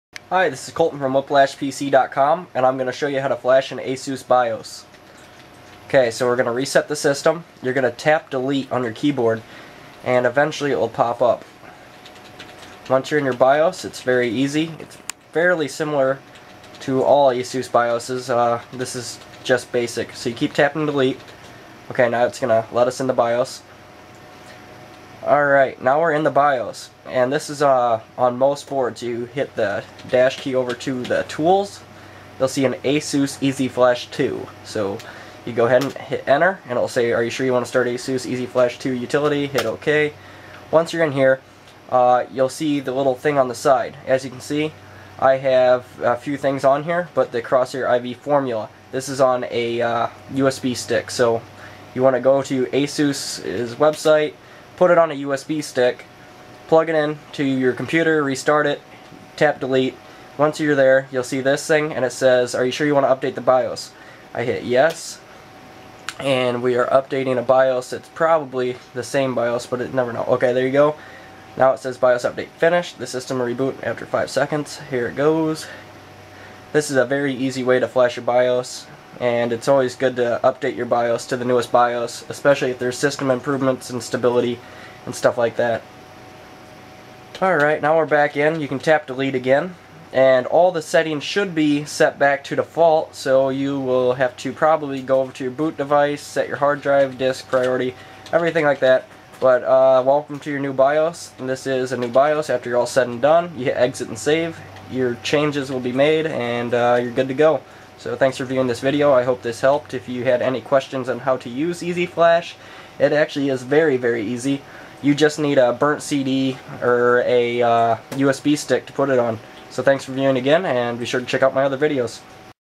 Guide : ASUS EZ Flash bios update
Today I'm going to show you how to update the bios on your ASUS motherboard using the ASUS EZ Flash 2 Method.

It's a very nice way of flashing your bios and I've never had any failure doing so.
The bios has a built in check to make sure the rom isn't damaged before flashing.

You can flash your bios by going to ASUS.com and looking for your motherboards model and downloading the bios to put on a USB device or CD to use with EZ Flash 2.

This particular flash was done on a ASUS crosshair IV formula Am3 motherboard however all asus motherboards have the same general EZ flash currently so you should be able to follow this method on most motherboards that have the ASUS EZ Flash option available.